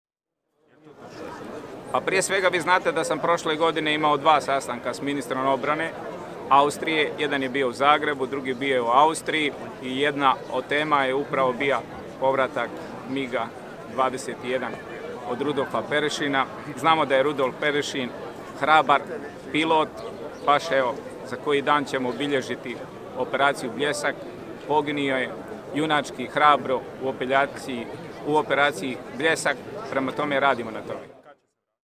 Izjava Damira Krtstičevića o MiG-u 21 Rudolfa Perišina
Split, 280419.
Izjava Damira Krtstićevića o Rudolfu Perišinu
Snimio: Tom Dubravec / CROPIX